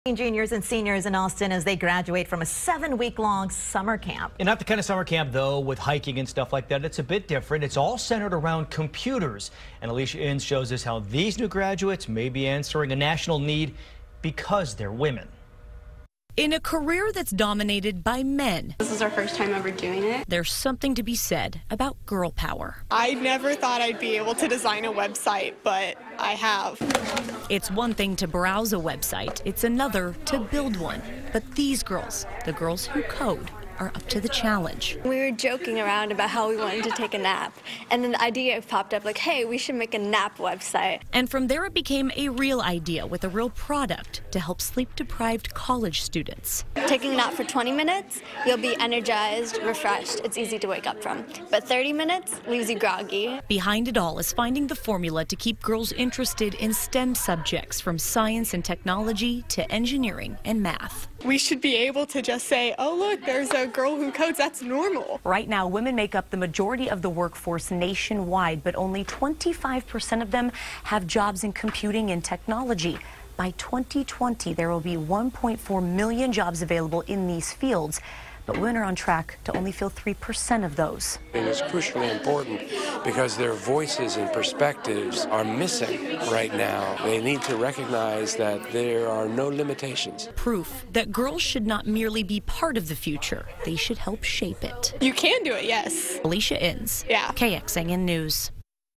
Girls Who Code: Getting women to love computer science
11th and 12th-grade girls are given 300-plus hours of instruction in web development and design along with robotics.